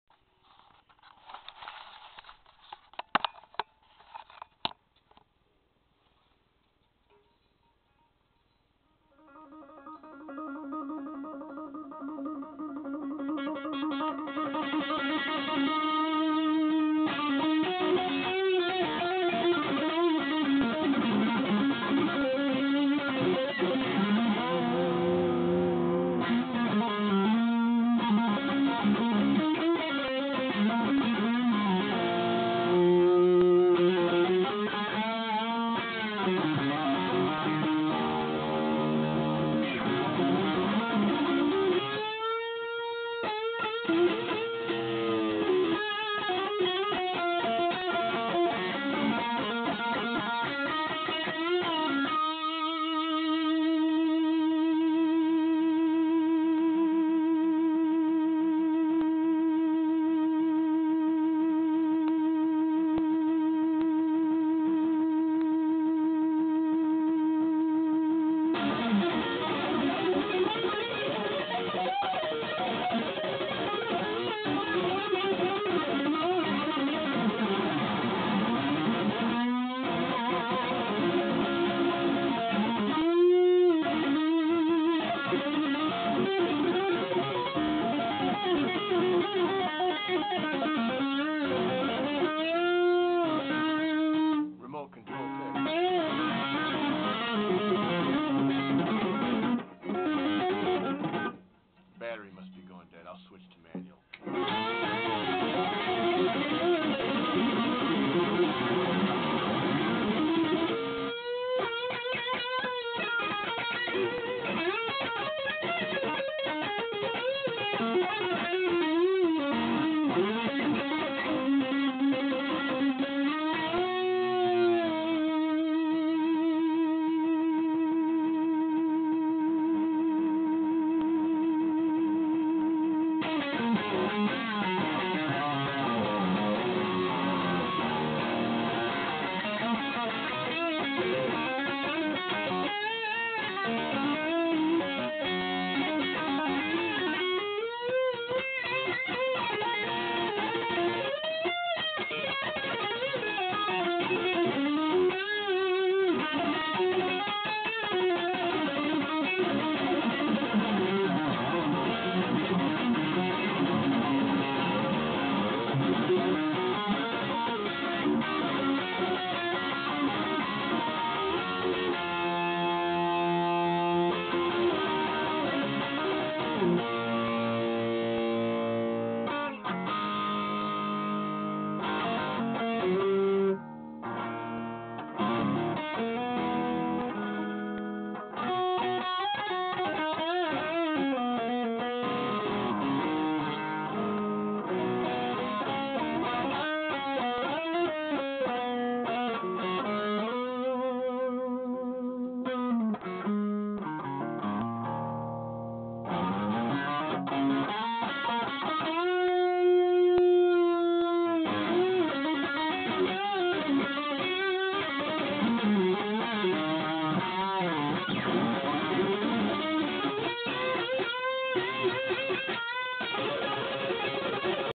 more sound checking
pretty much established that the sound is ok,  but the light is bad so we'll do the real deal upstairs . .